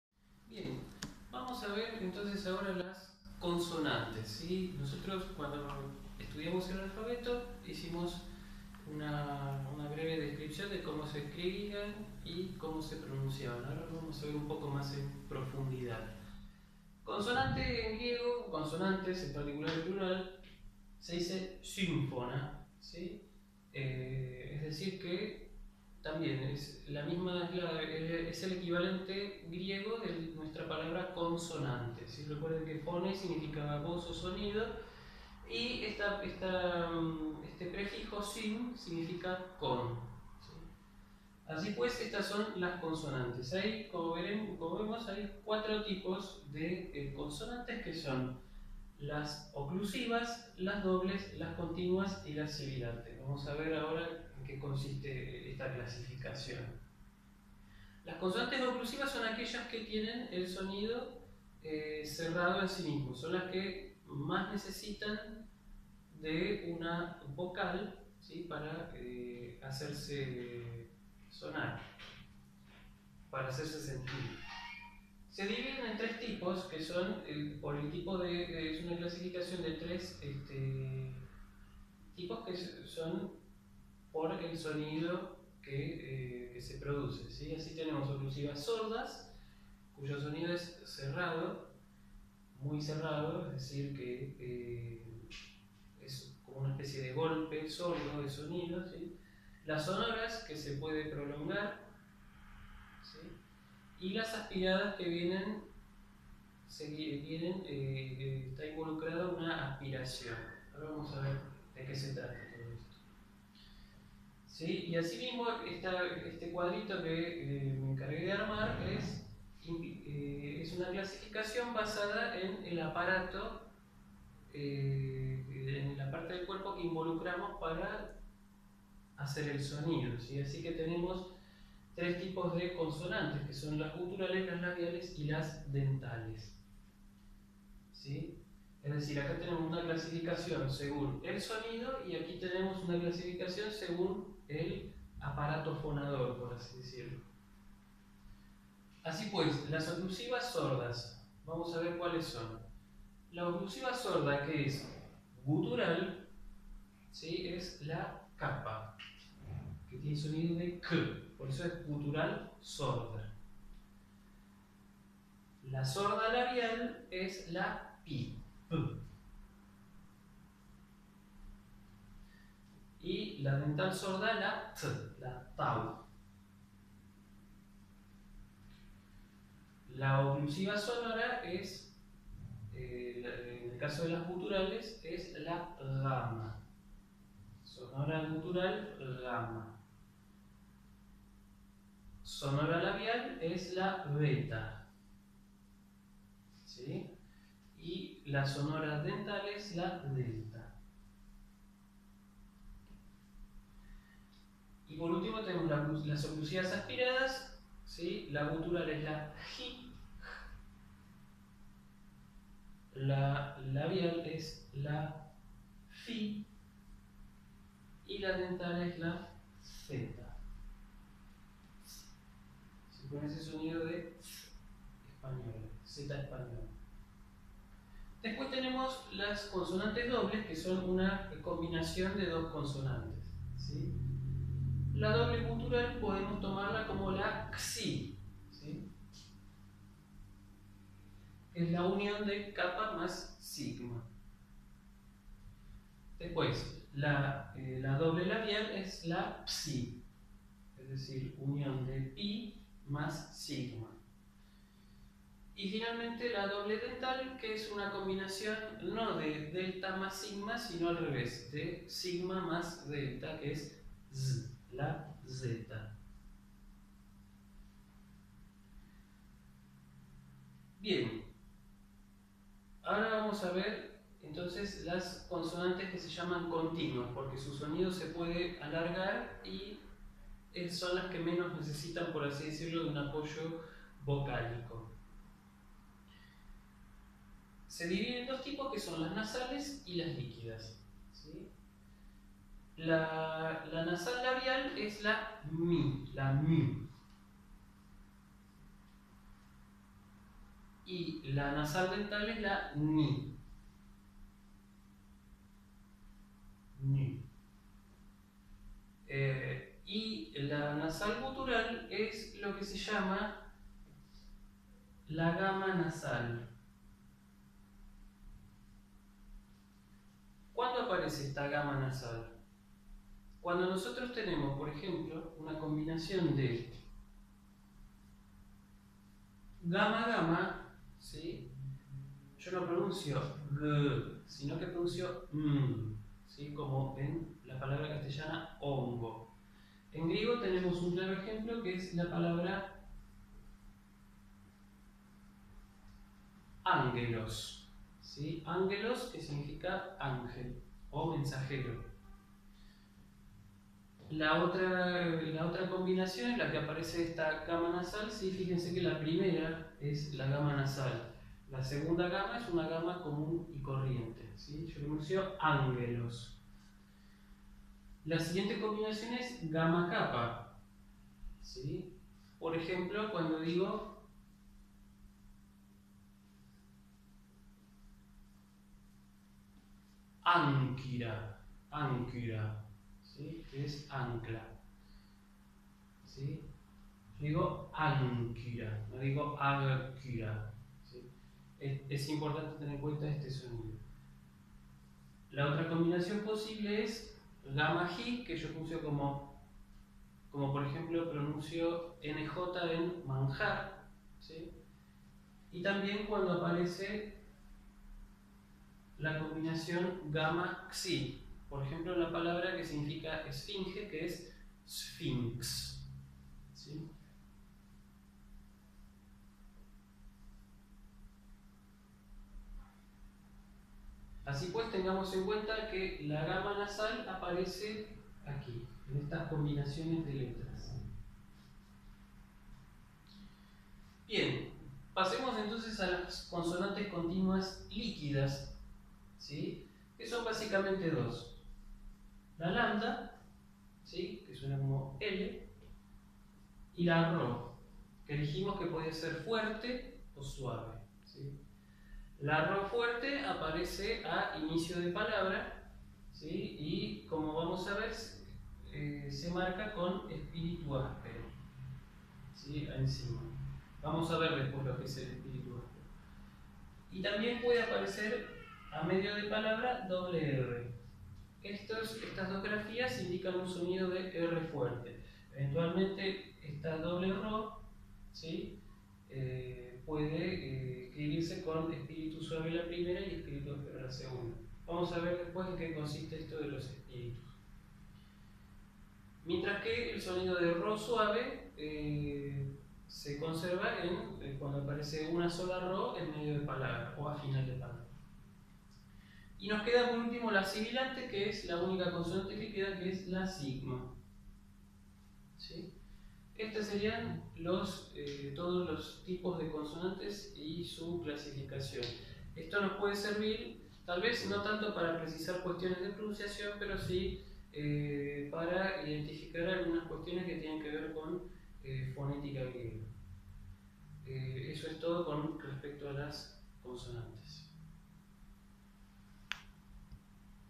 Material Complementario Lengua y Cultura Griega I - Consonantes
Introducción a la lengua griega: consonantes. Video grabado como material complementario de la cátedra Lengua y Cultura Griega I, bajo idea y dirección del alumno Sebastián Lamonega, grabado en 2017.